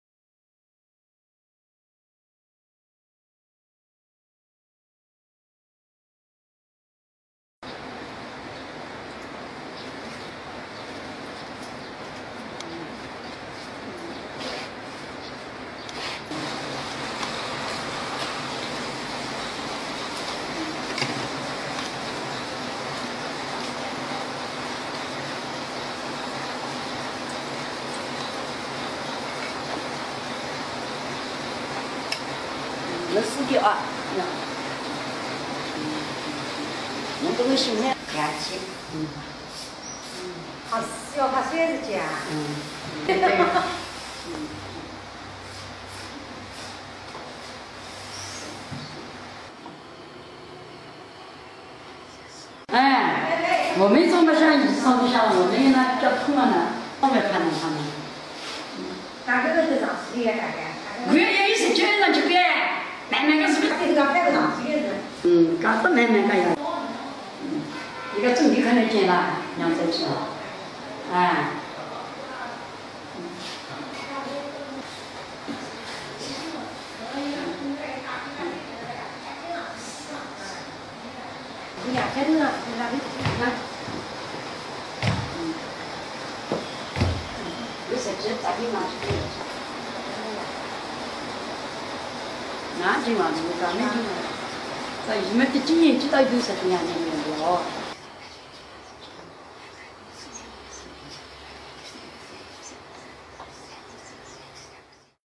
Impressive workout routine of an 85-years-old woman
Video from China shows the impressive workout routine of an 85-years-old woman!